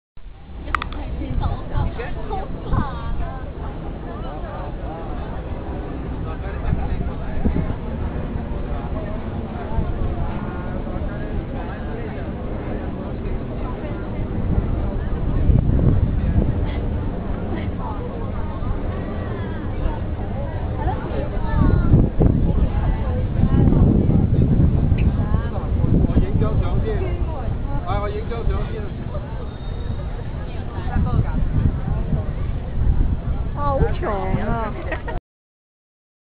Wonderful habrbour view
Such nice view in this visit. just passed my first ever operation ... going slow in this mini-trip with my mom and lovely friend in HK.